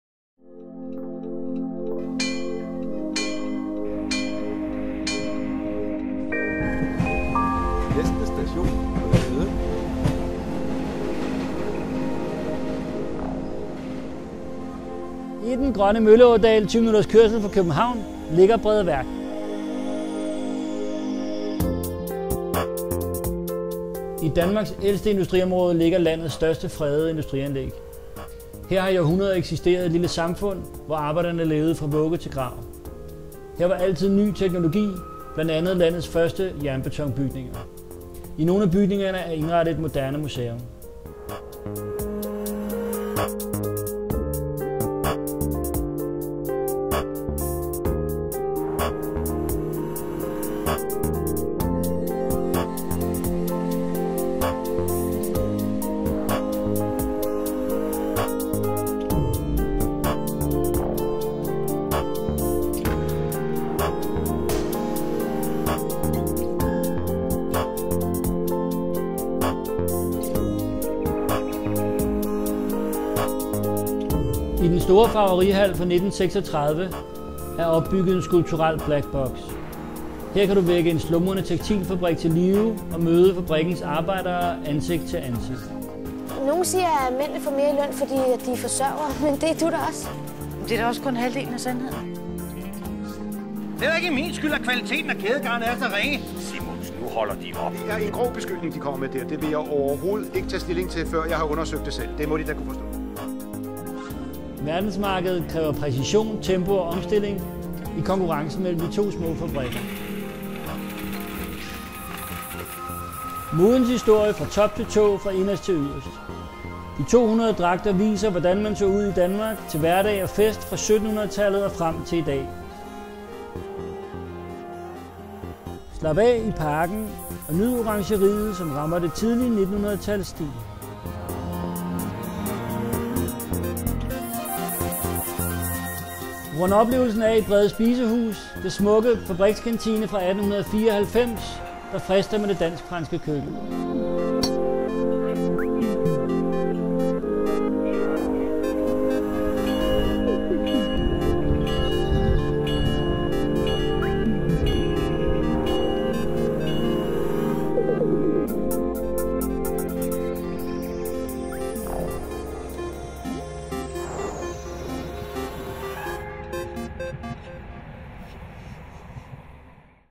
Brede Værk - industrikultur og 200 års mode
Mikkel Thelle fortæller om museet og aktiviteterne i udstillingerne. Få en formsag på hvad du kan opleve på Brede Værk - Museum for Industrikultur.